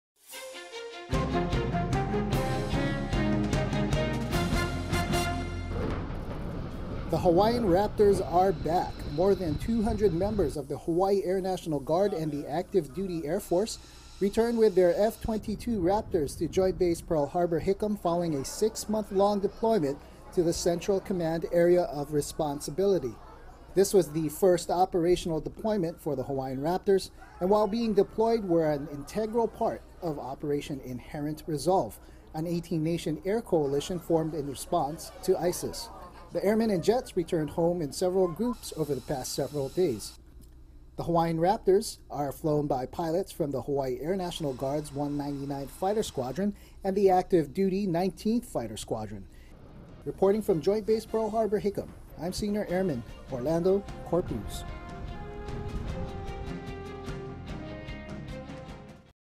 Minuteman Report - Hawaiian Guard Raptors Return Home
The Hawaiian Raptors returned home from a six month deployment to CENTCOM AOR. This was the first operational deployment for the Hawaiian Raptors.  Senior Airman Orlando Corpuz reports.